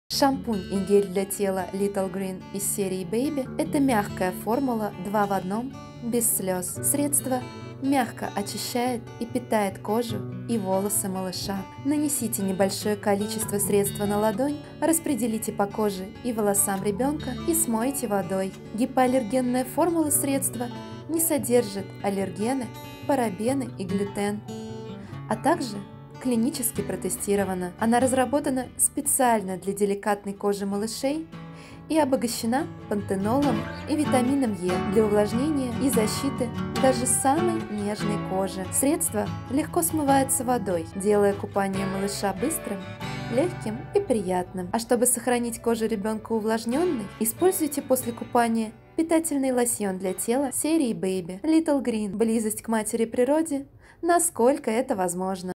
Шампунь и гель для тела для детей от 0 мес Little Green / Shampoo and Body Wash Baby
Бережное средство 2-в-1 позаботится о первых волосиках и нежной коже малыша. Экстракт алоэ, витамины B5 и Е обеспечивают самый мягкий уход с первых дней жизни. А особый аромат успокаивает кроху и готовит ко сну.